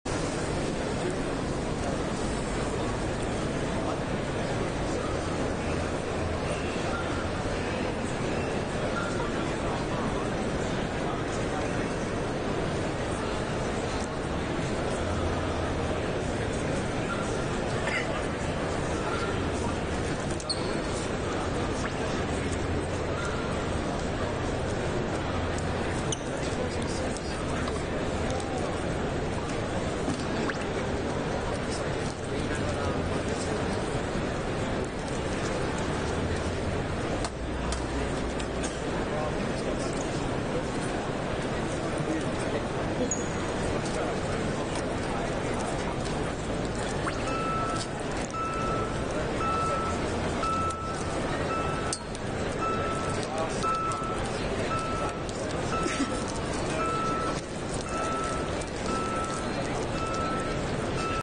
"panavi" - Digitally-Assisted Cooking with projector / instrumented frying pan - CHI2012
Took this video from the demo floor at CHI 2012. A projected timer on a stovetop with other digital cooking assists (inc. a temperature sensing frying pan)